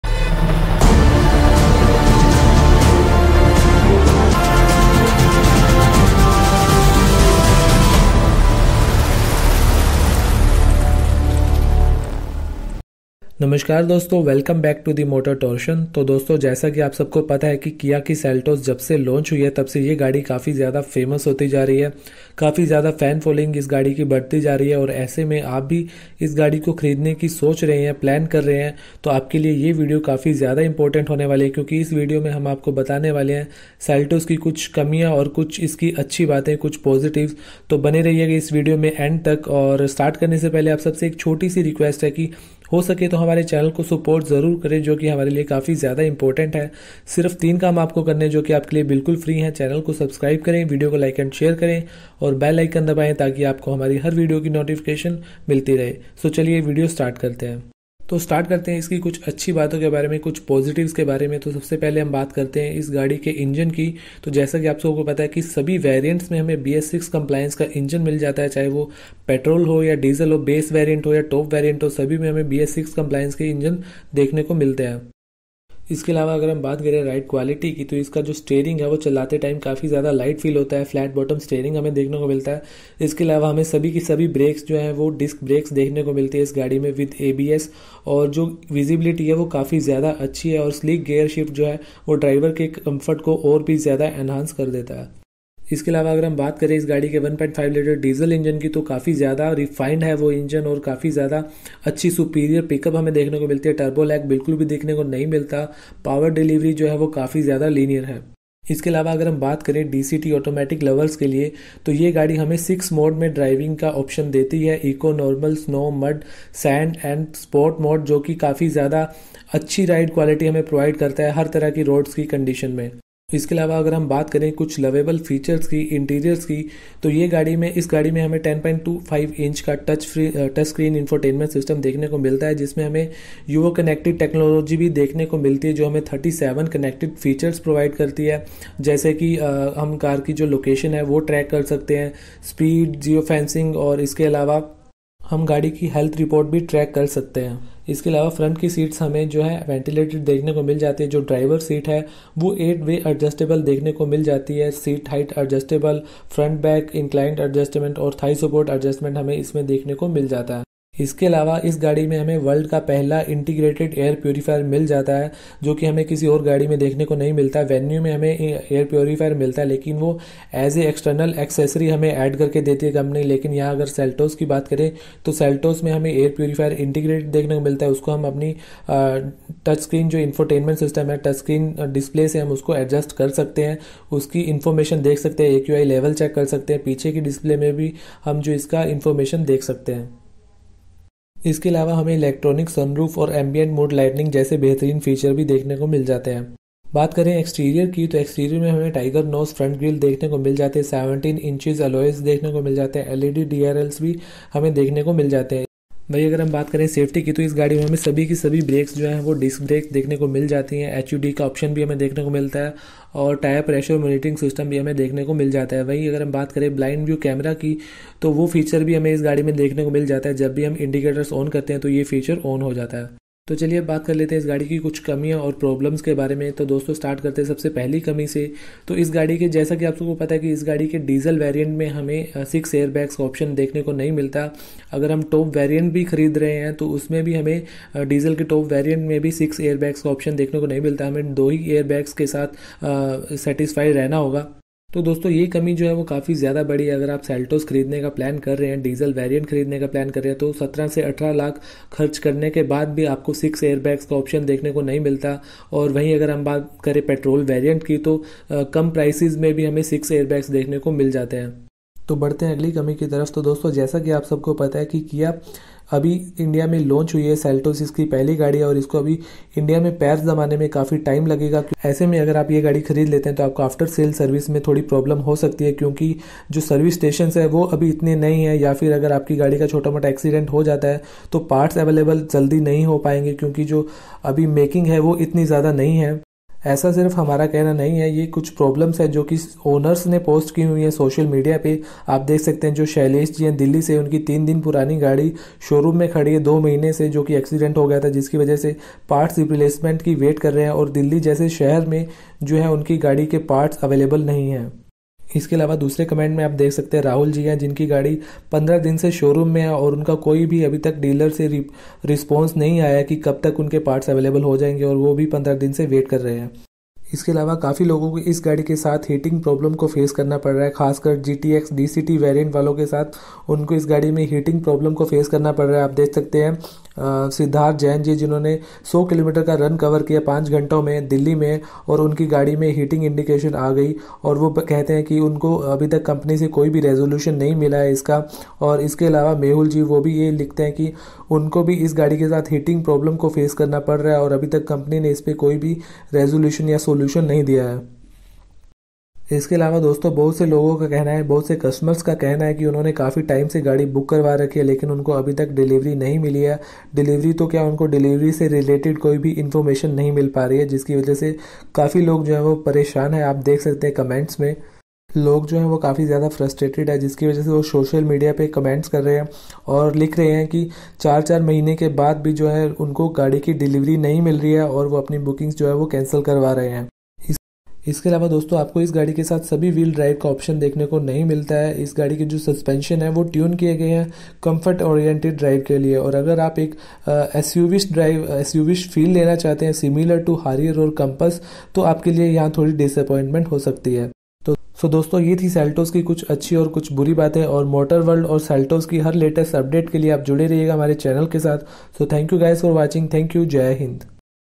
Kia Seltos Negatives | Why not to buy seltos | Seltos cons
Seltos Negatives and Positives | Why not to buy seltos
Inside Video:
In this video we have explained some bad things about kia seltos, some negatives of kia seltos and seltos problem, and tried to explain seltos problem in details.